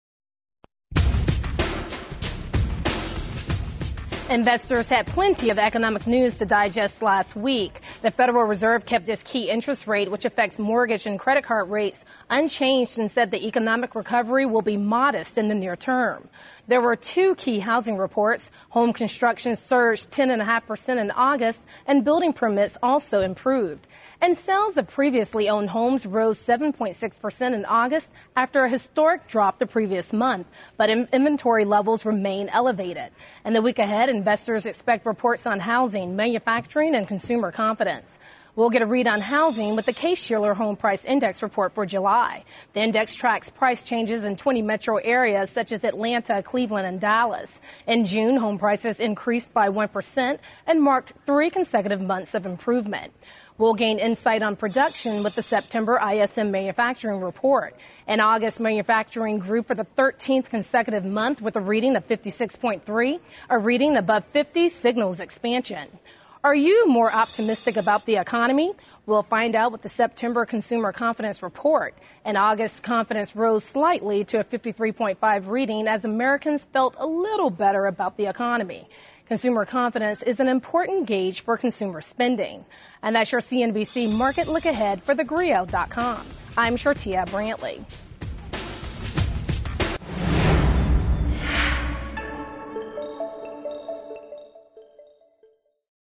Latest economic news for investors- 9/27/10
Investors had plenty of economic news to digest last week, the Federal Reserve kept its key interest rate-which affects mortgage and credit card rates-unchanged. Index reports are due on home price and tax prices are changed in 20 Metro area's.